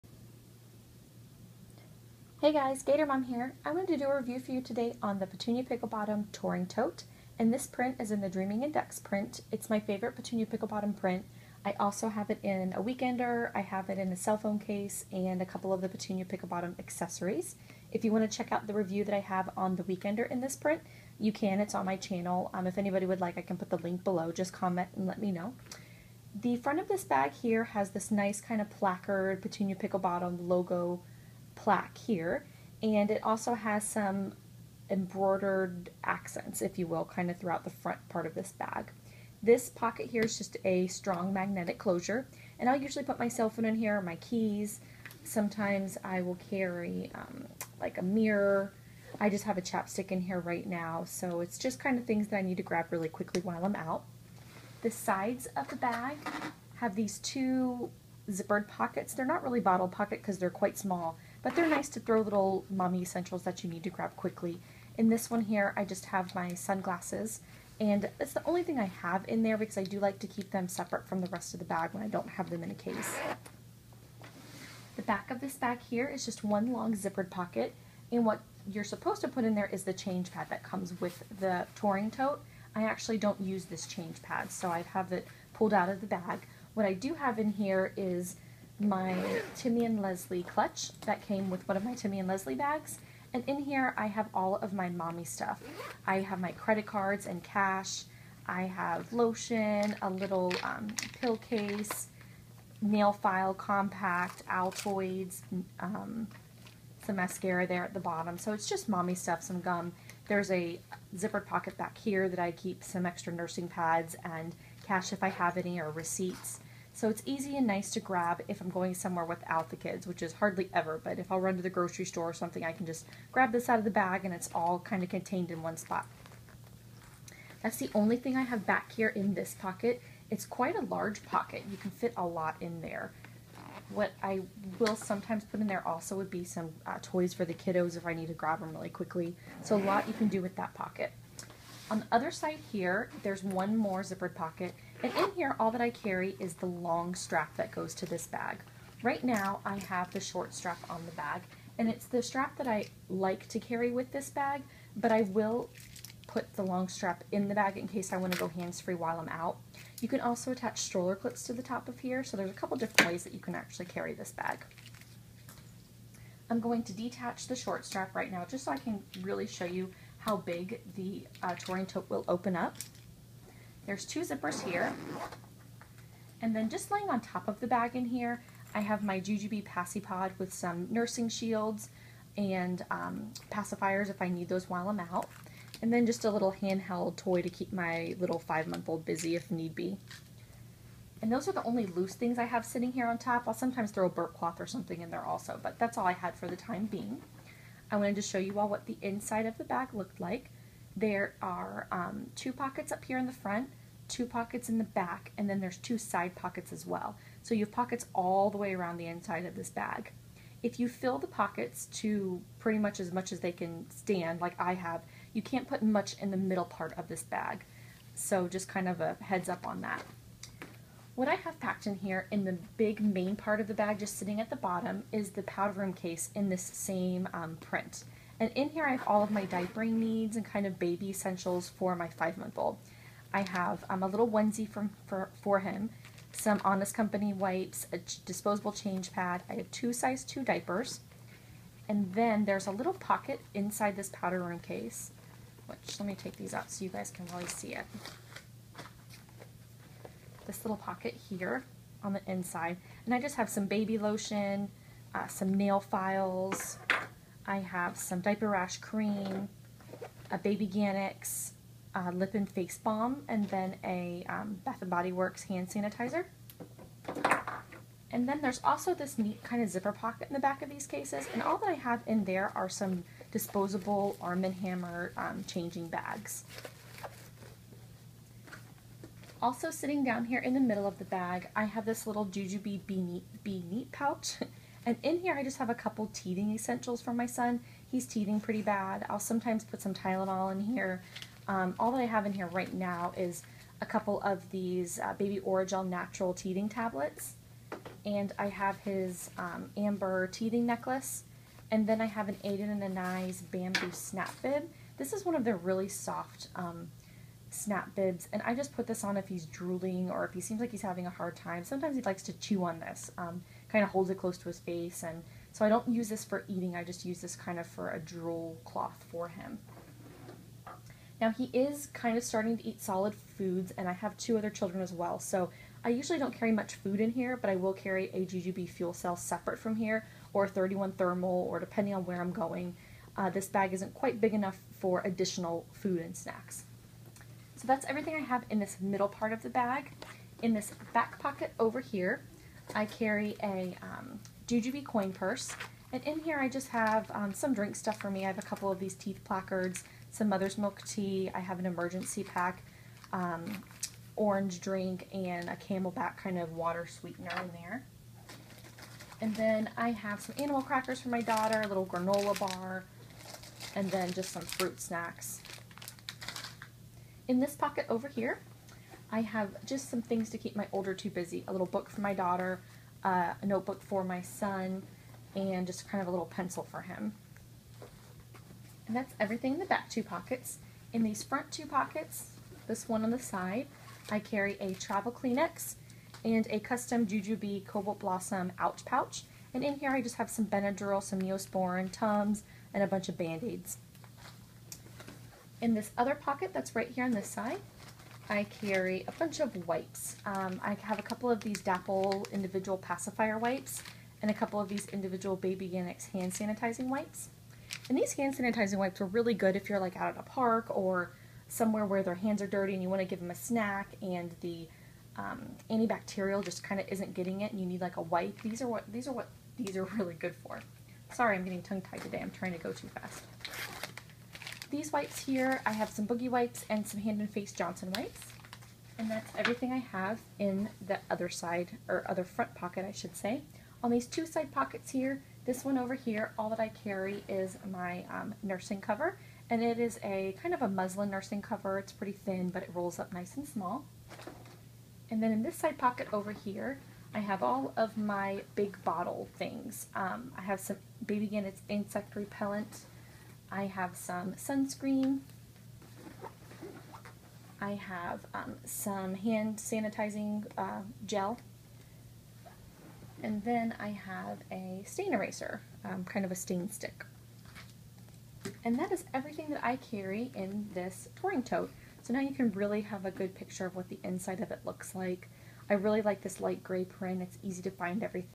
Packing video for my Petunia Pickle Bottom Touring Tote in dreaming in dax print
This is a packing video and review on the petunia pickle bottom touring tote in dreaming in dax print.